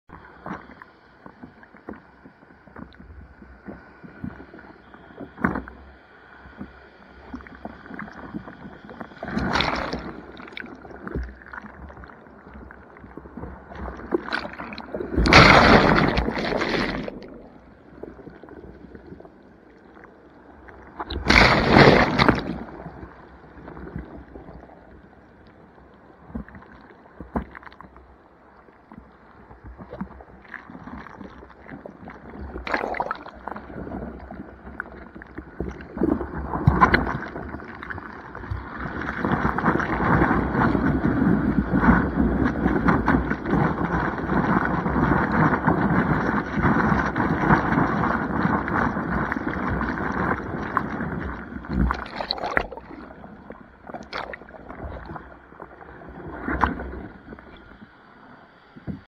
boogieboarding 5 19 22
First time in the water in 2022